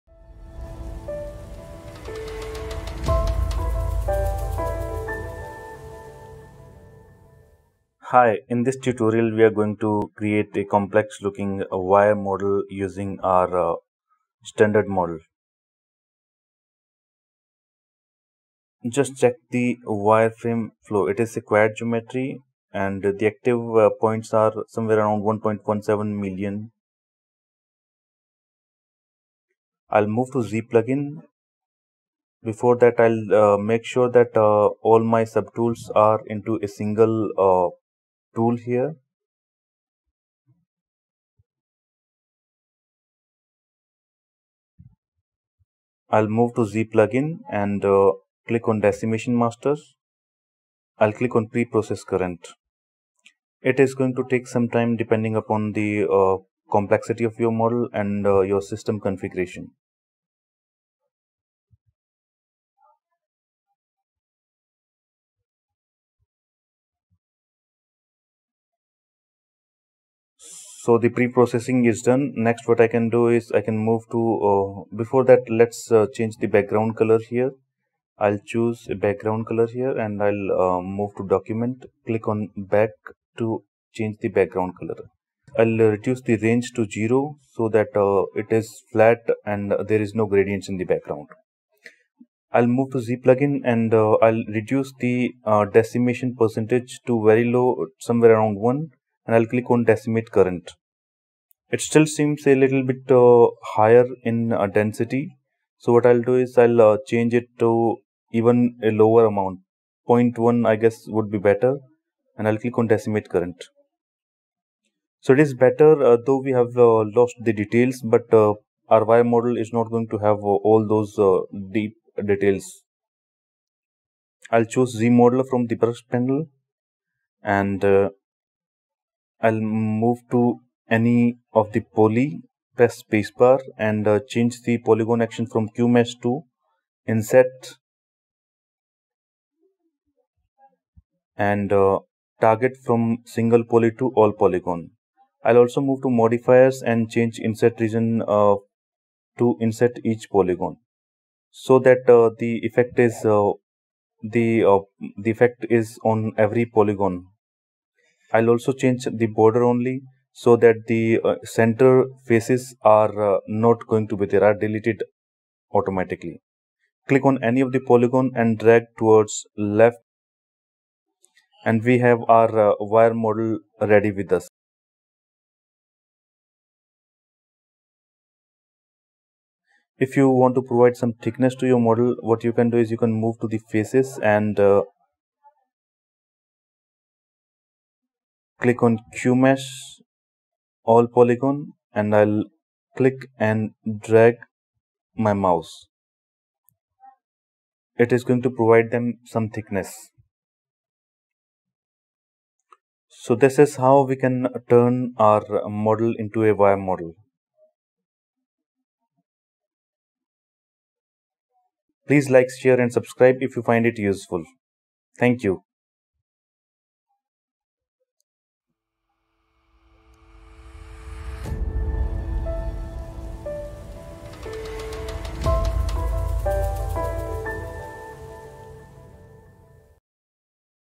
ZBrush Tips and Tricks: Quick Wire Model From An Existing Model
This tutorial will give an idea about converting an existing model into a complex looking wire model using ZBrush.